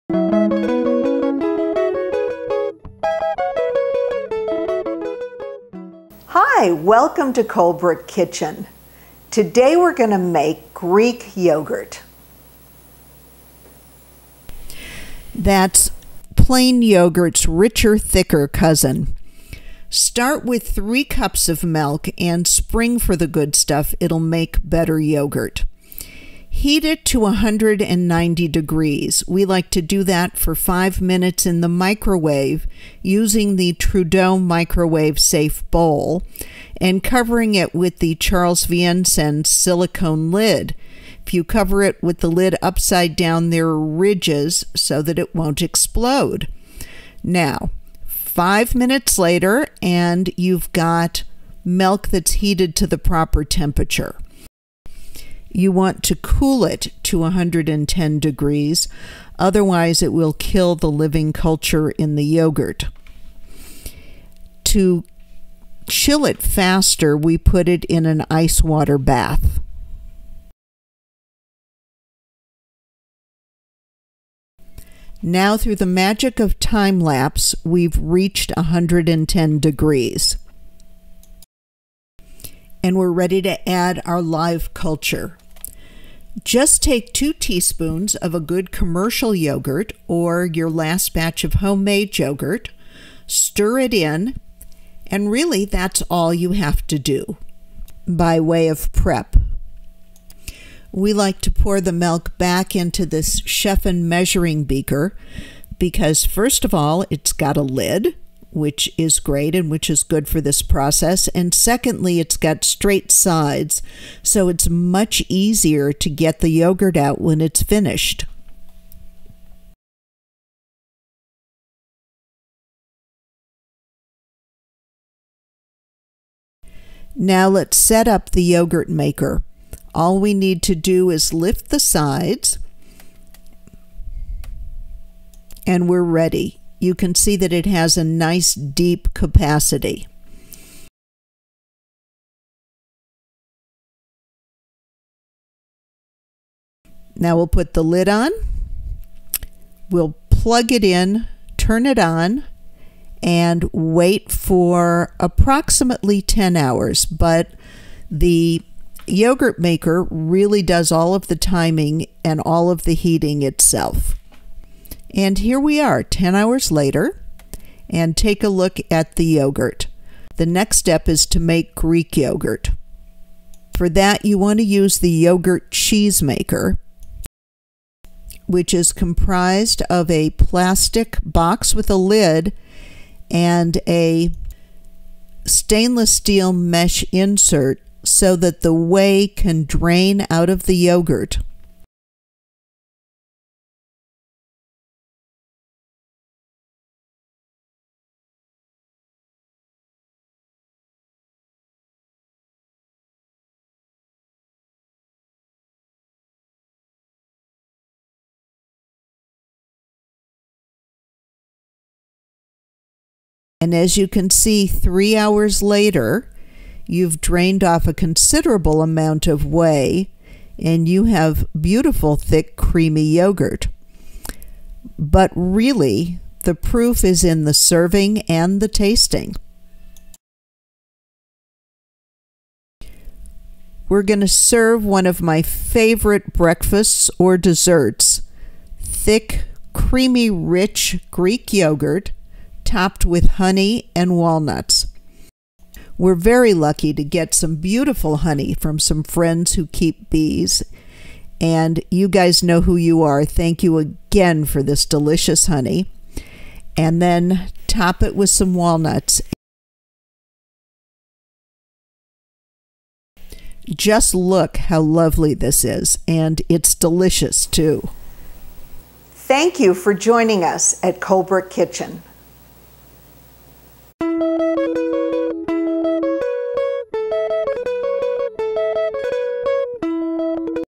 It's easy to make homemade yogurt with the Cuisipro Yogurt Maker
See the step by step process to make homemade yogurt, and then turn it into Greek yogurt.